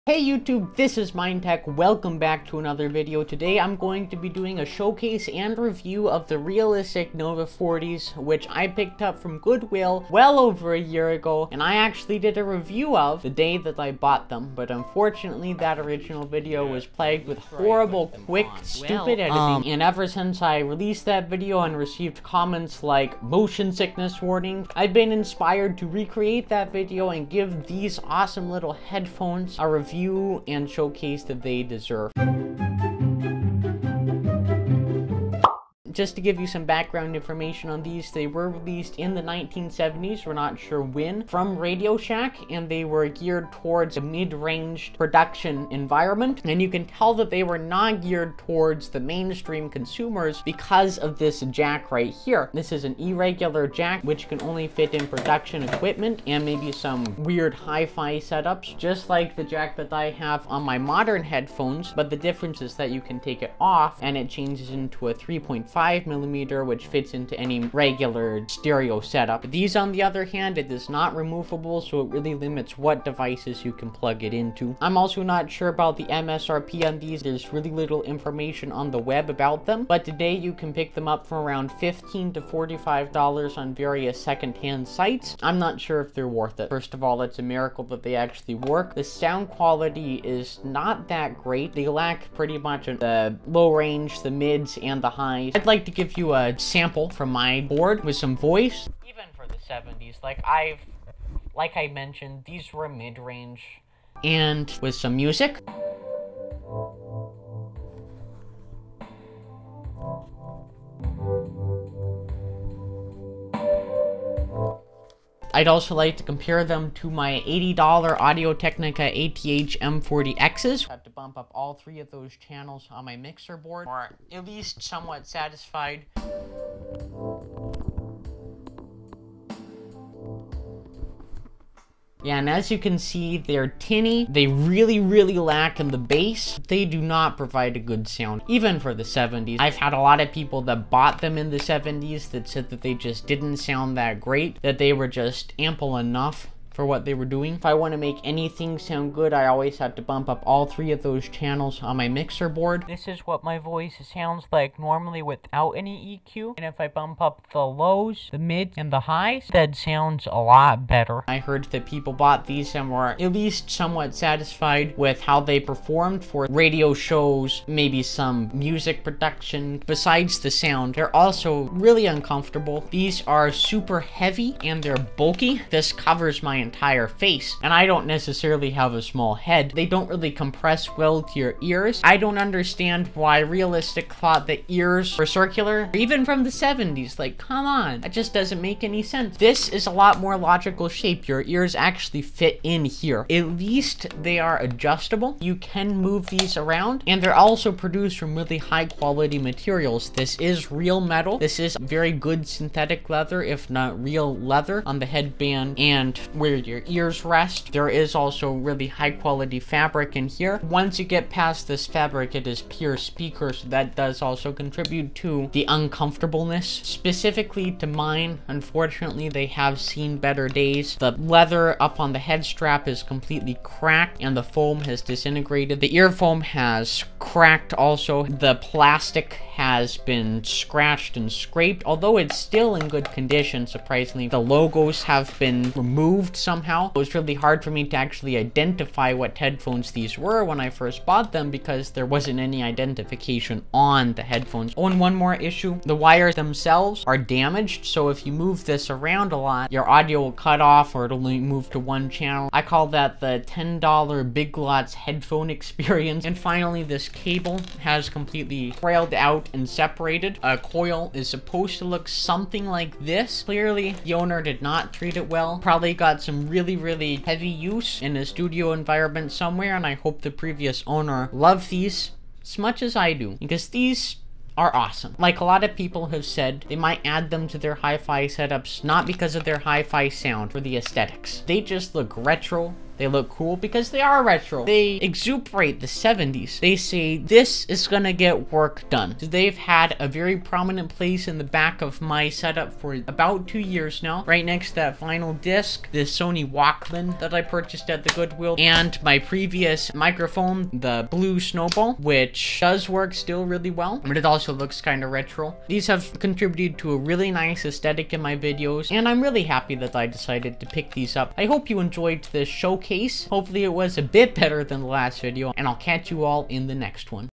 RETRO Realistic Nova 40s Showcase and Review! (from RadioShack!)
I made a first-impressions video of the Realistc Nova 40s a year and a half ago right when I brought them home from the goodwill.  Unfortunately, some people were expecting a different style of video - and I have been wanting to deliver that for a long time!  This video serves as a showcase of what these headphones are, how they were used, and how I use them today.  I provide audio samples, video shots, and commentary based on the research I have conducted.  Also, I give my opinion on whether or not you should pick these up on a second-hand site to add to your retro audio collection.  Hope you enjoy!